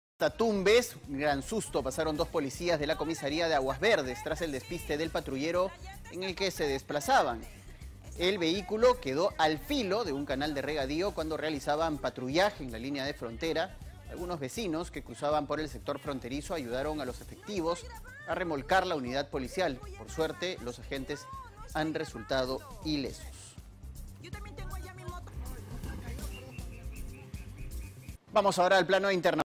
Tumbes: patrullero se despista y casi cae en quebrada
En Tumbes, un gran susto pasaron dos policías de la comisaría de Aguas Verdes tras el despiste del patrullero en que viajaban.

Tras el accidente, el vehículo quedó al borde de un canal de regadío cuando realizaba patrullaje en línea de frontera.

Algunos moradores que cruzaban por el sector fronterizo, ayudaron a los dos efectivos a remolcar la unidad policial.